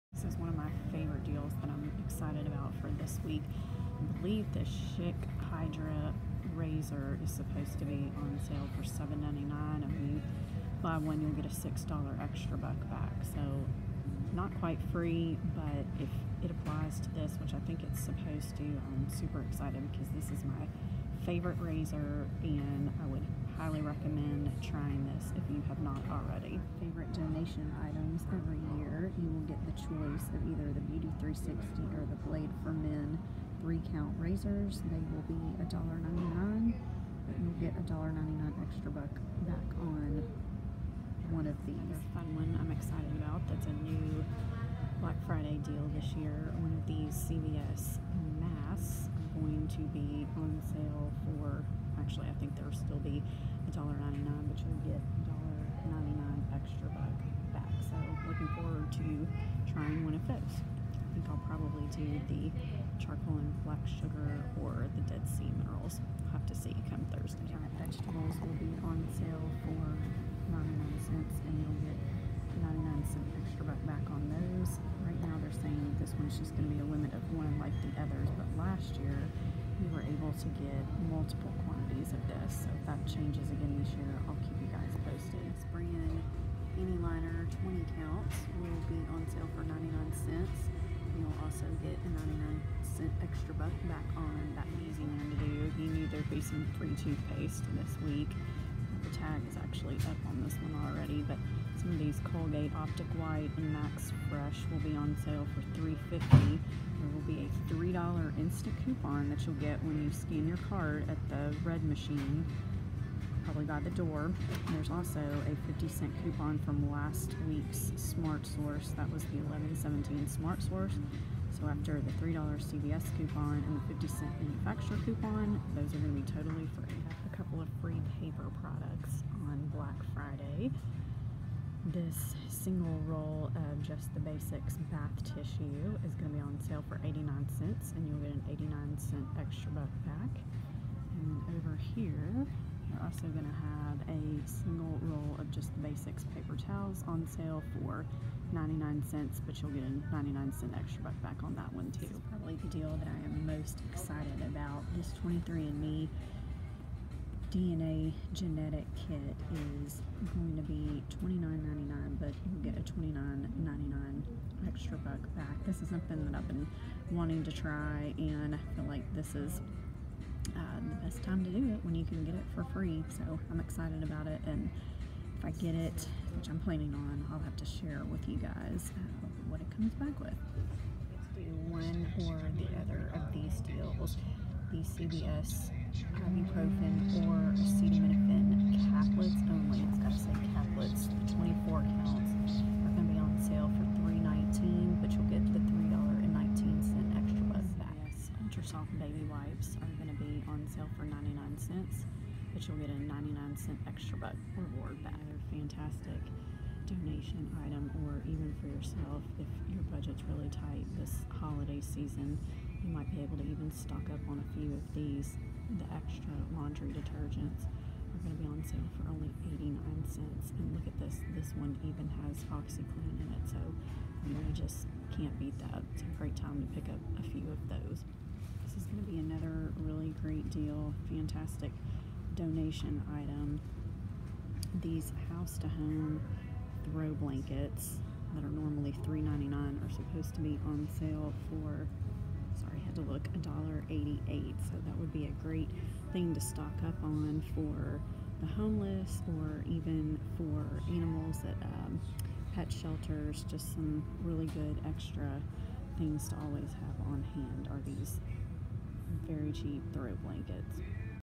CVS IN STORE BLACK FRIDAY PREVIEW - STARTS THANKSGIVING DAY! - FREEBIES GALORE INCLUDING 23 & ME!
Come with me in store to get a heads up on some of the best Black Friday deals at CVS.